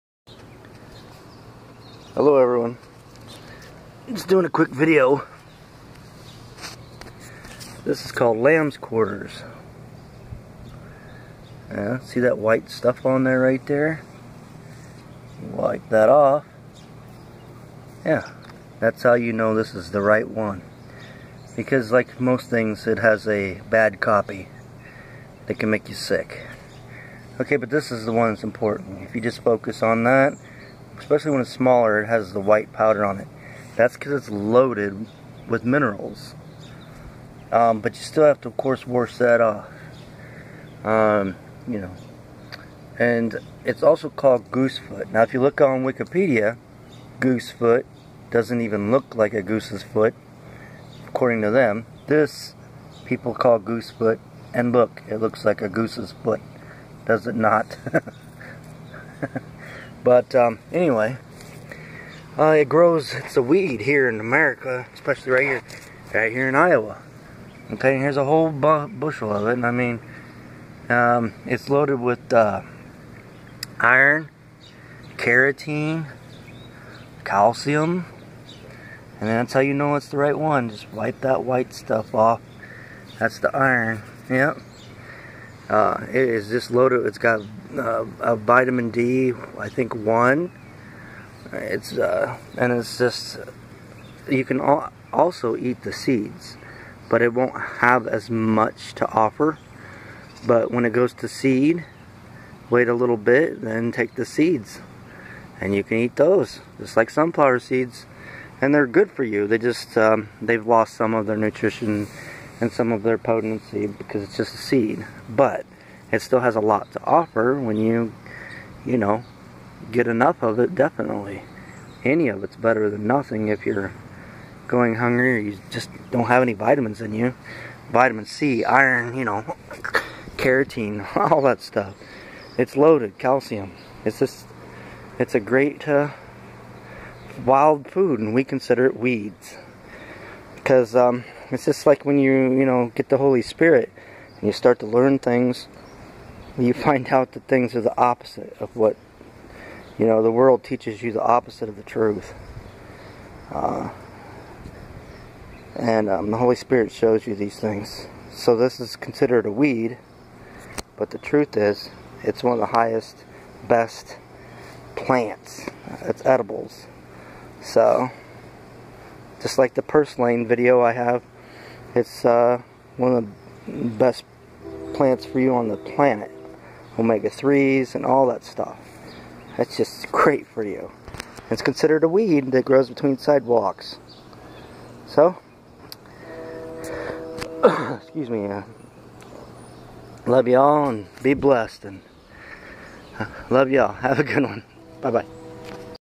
Ediable weed (Lambs Qaurters) I'm Muscatine Iowa .
It's a wild edibiele plant that most in the world call a weed . This weed has , carotine, calcium, vitiman D1 , and iron and more . Loaded with minerals . It's not a weed but a food source.